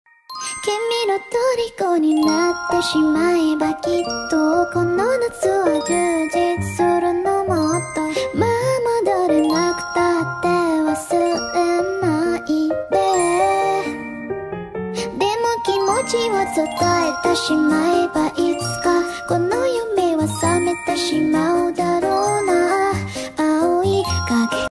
EP 10 How you know your cat is asking for play?
This cat want to play with me.